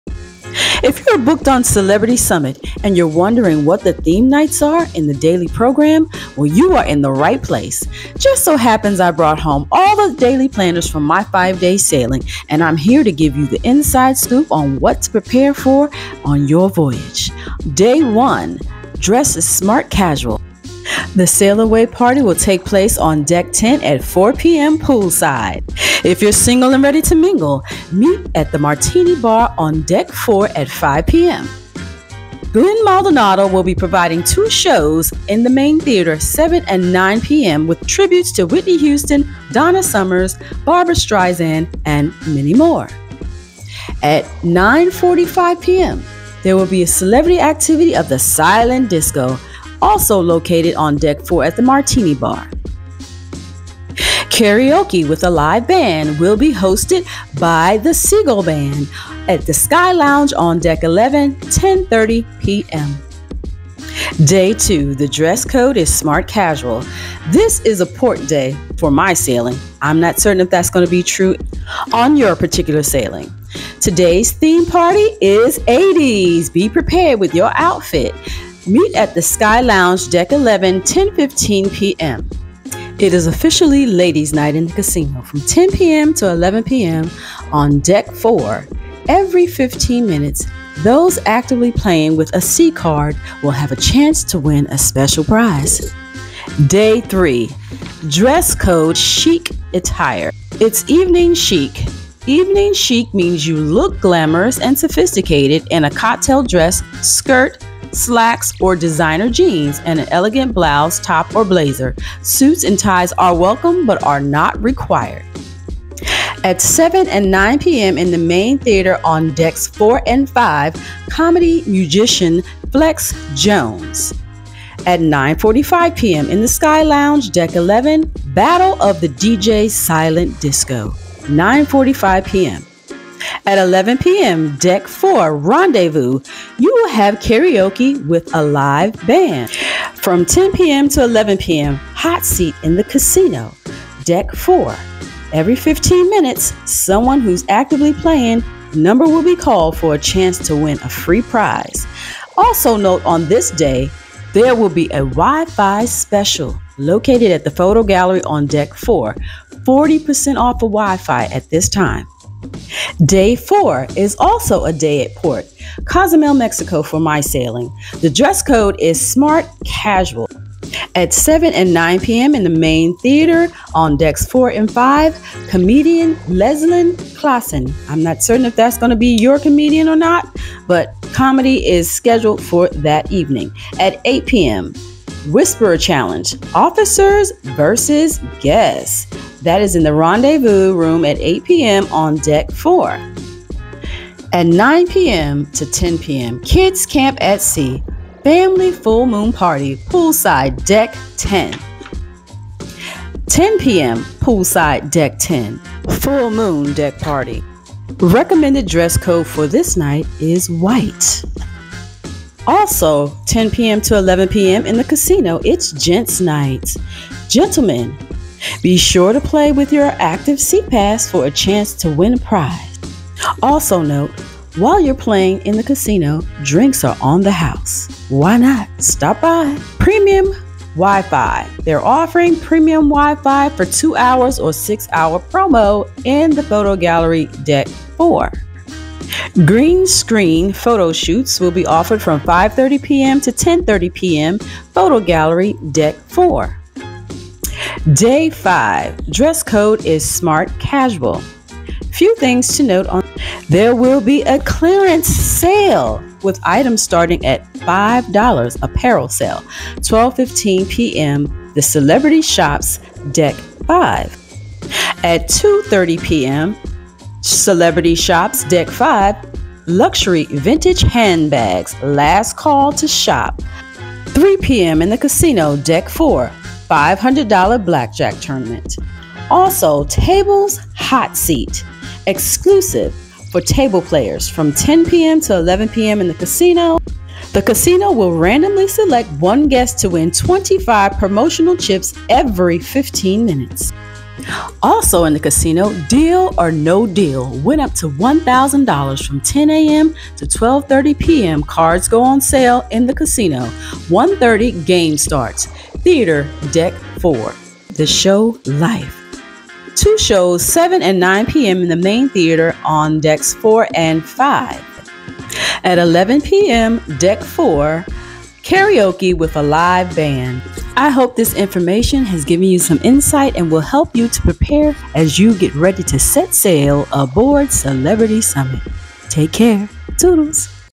CELEBRITY SUMMIT DAILY PROGRAM REVEALED!🥳😎😉
Celebrity Summit DAILY PROGRAM REVEALED. This video is a must see to help you pack and prepare for all of the theme events onboard. In this video, we're revealing the daily program for the Celebrity Summit.  Find out what will happen before you set sail.  I'm excited to announce daily events for Celebrity Summit! If you're planning your cruise on the Celebrity Summit daily planner| what to expect,  you need to watch this video! In it, I discuss the daily events for the cruise and give you a taste of what to expect! This video has it all! So get ready to have the time of your life on the Celebrity Summit! I hope you enjoy this video and find some useful information!

celebrity summit
celebrity summit daily program
first time celebrity cruise Themes
On the record with Ronda